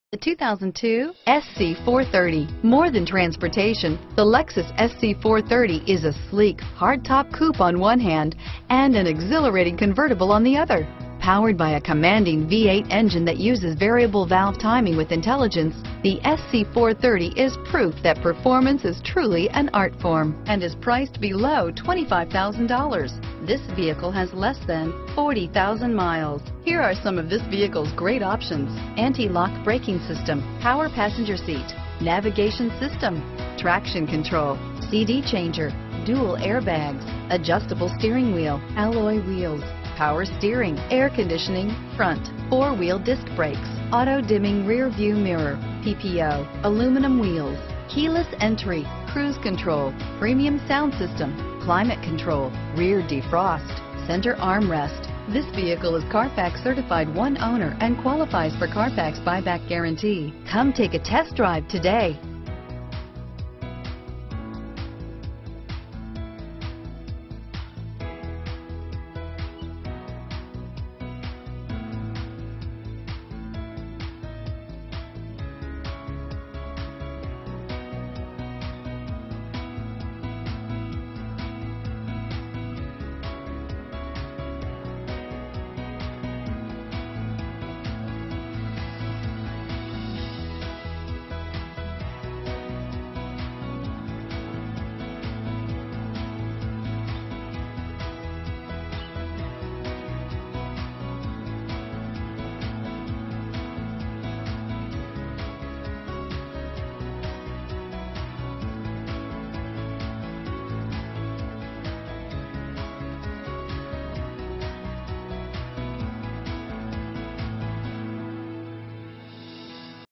2002 Lexus SC 430 used, Long Island, Huntington, East Northport, Plainview, NY AU4284
2002 Lexus SC 430 430 - Stock#: AU4284 - VIN#: JTHFN48Y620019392

363 E. Jericho Turnpike

CLEAN CARFAX, Dealer Serviced!!, NAVIGATION, and ONE OWNER!!. Lots of Luxury! Indulge your senses! Confused about which vehicle to buy? Well look no further than this wonderful 2002 Lexus SC. J.D. Power has named the 2002 SC as the highest ranked in Overall Performance and Design in its class. Zooooooom! A powerful machine right here. You gotta drive it! It's unbelievable how it moves you. really!